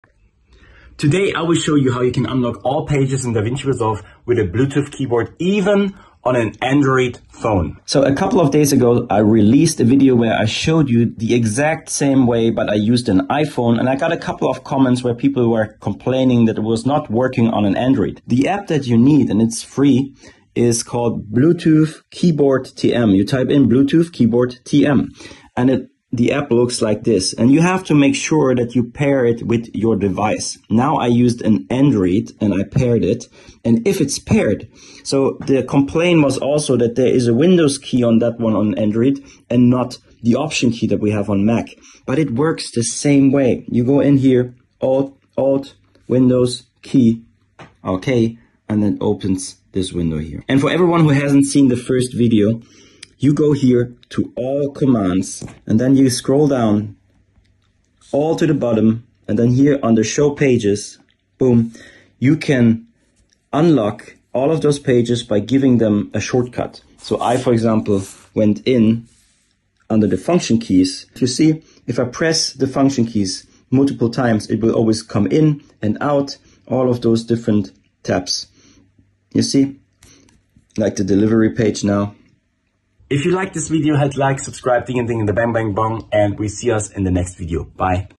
FULL DaVinci Resolve iPAD - with ANDROID Bluetooth Keyboard TUTORIAL
FULL DaVinci Resolve iPAD - with ANDROID Bluetooth Keyboard TUTORIAL - A couple of days ago i published a video where i showed you how you can unlock all Pages in DaVinci Resolve for the iPad Pro. Including the EDIT Page, Fusion Page etc. In this Video i showed you a way using no external Keyboard. Rather a bluetooth App on my iPhone 13 Pro. Some people with Android had problems. So today the same solution on an ANDROID phone.